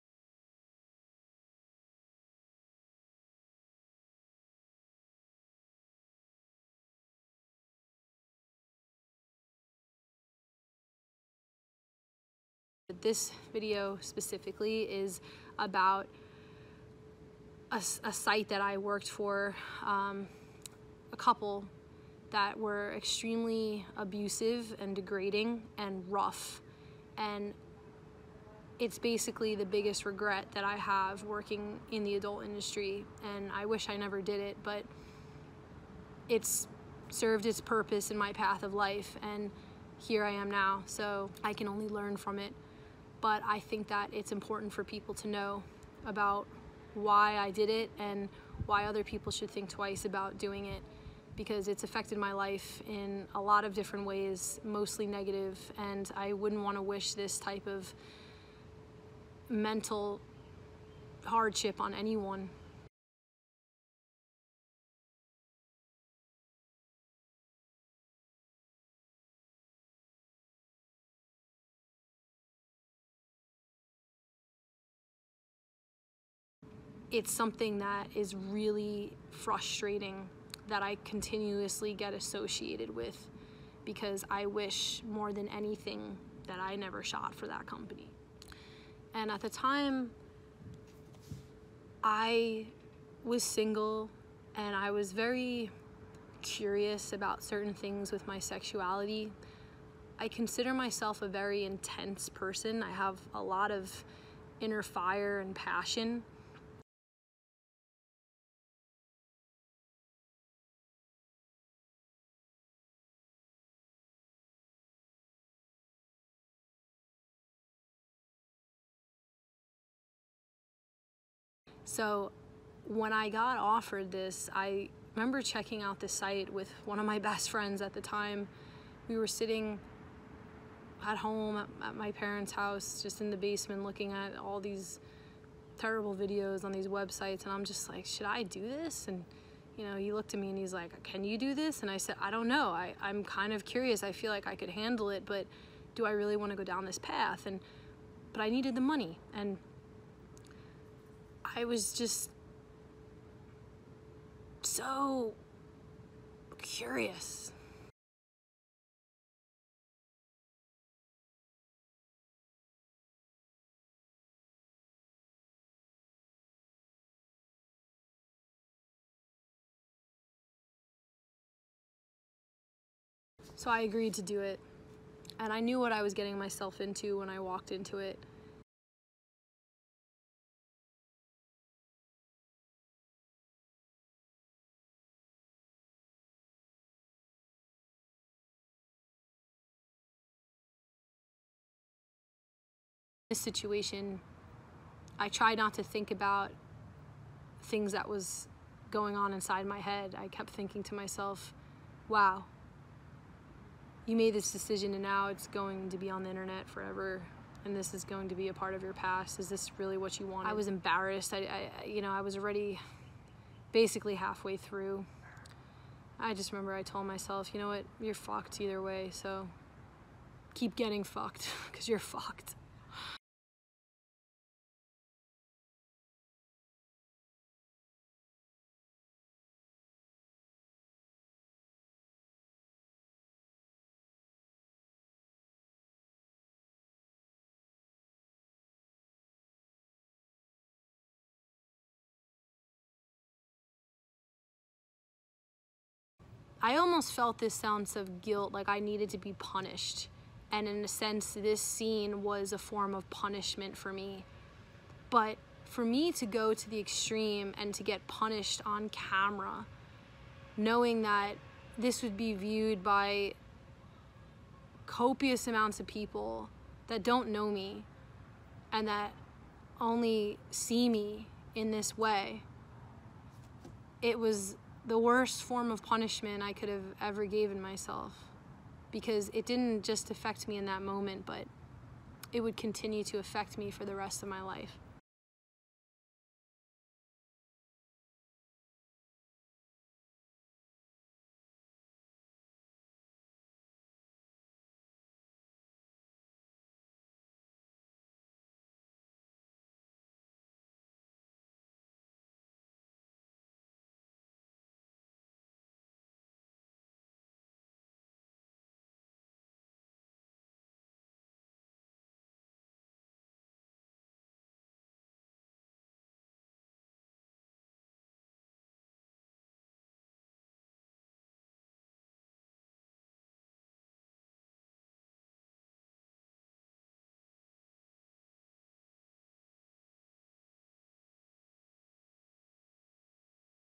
[Redpill] The Regret... of Felicity Feline.
Today we take a look at the regret of former adult film star, Felicity Feline.